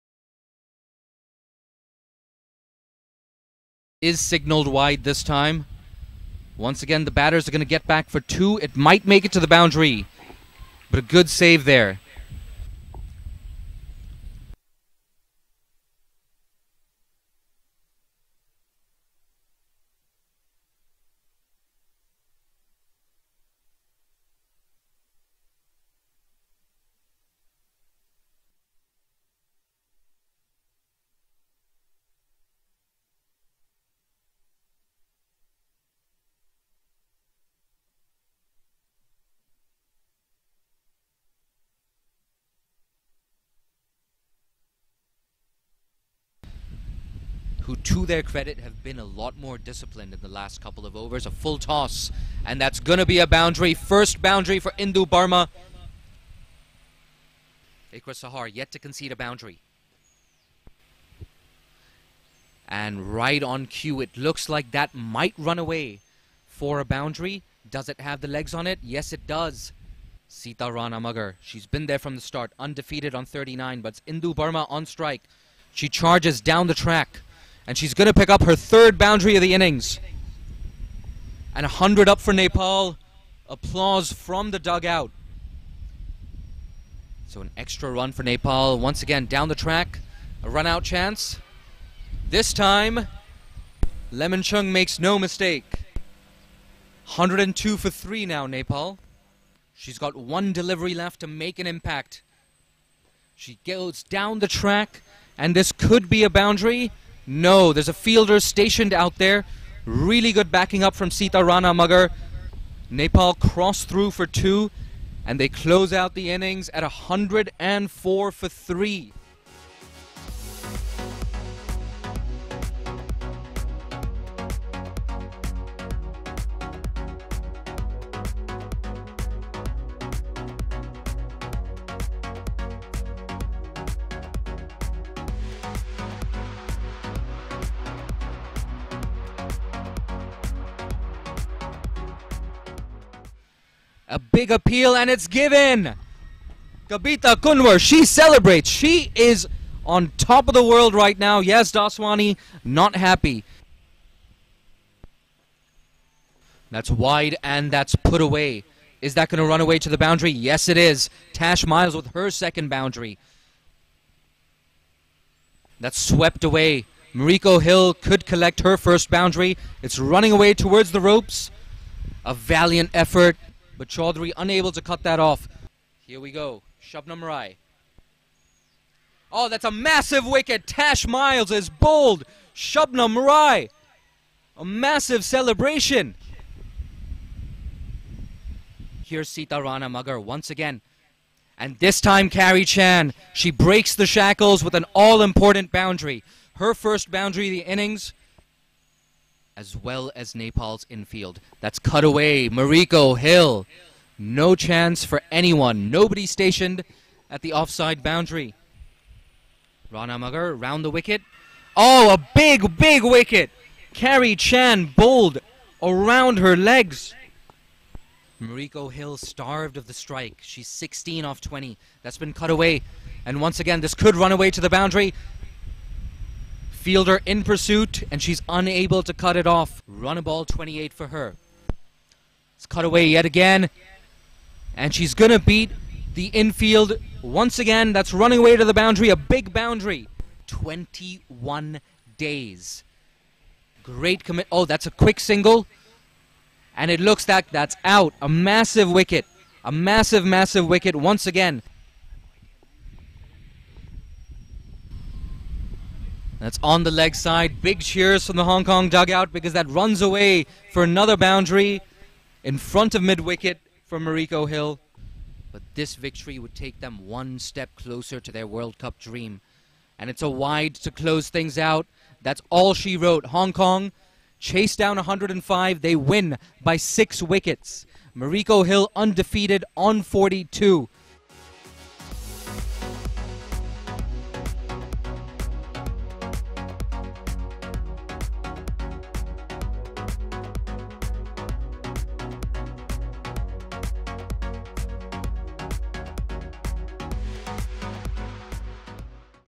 Highlights - HK Women vs Nepal Women - ICC Women's T20 World Cup Qualifiers Asia Region
The Hong Kong Women's team kicked off their T20 World Cup qualifying campaign with a win against Nepal yesterday, beating the highest-ranked team in the ICC CWC Qualifiers Asia Region! This was also the first time HK Women have beaten Nepal Women in T20Is, and was also the 1000th Women's T20I ever played! Catch the highlights from the game below, and be sure to follow all the action from the Qualifier live on ICC.tv and FanCode!